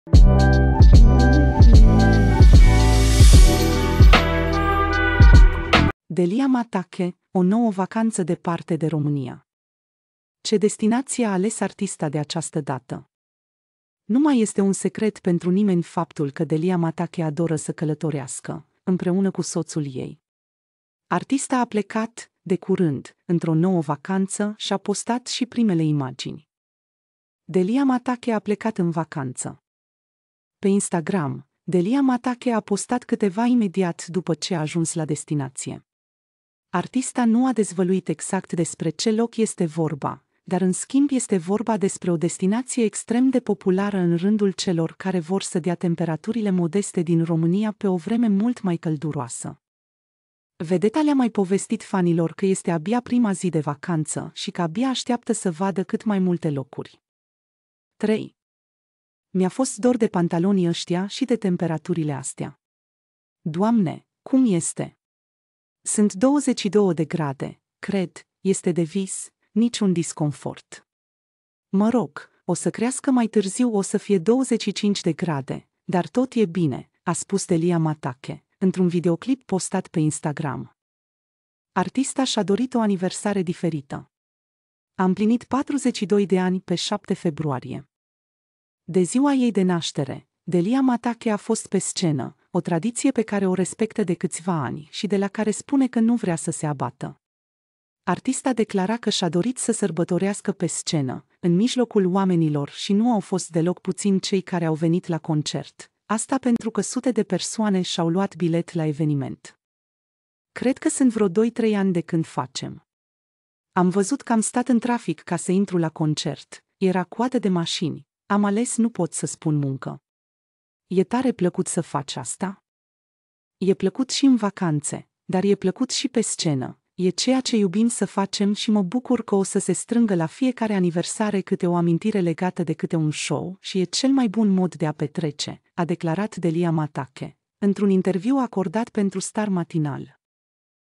Delia Matache, o nouă vacanță departe de România. Ce destinație a ales artista de această dată:...
După ce, în urmă cu câteva zile, și-a sărbătorit ziua de naștere pe scenă, Delia Matache a plecat într-o nouă vacanță. Artista adoră să călătorească, așa că, a dat temperaturile modeste din România pe vremea mai călduroasă.